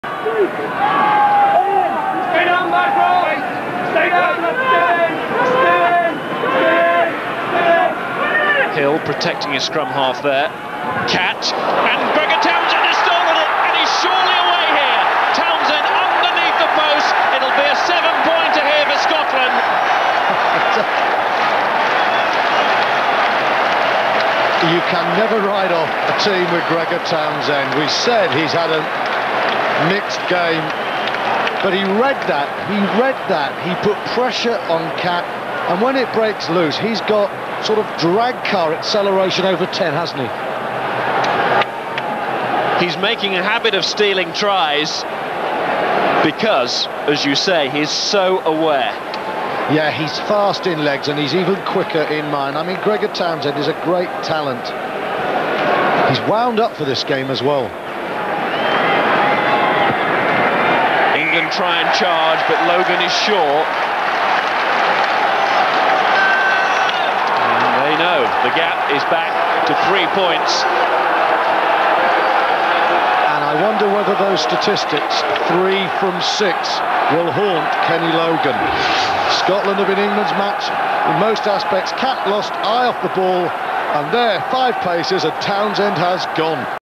Gregor Townsend pilfers Mike Catt for runaway try
Scotland fly half Gregor Townsend puts pressure on England fly half Mike Catt and then pilfers the ball after a fumble and runs away to score under the posts during the 5 Nations match at Twickenham in February 1999.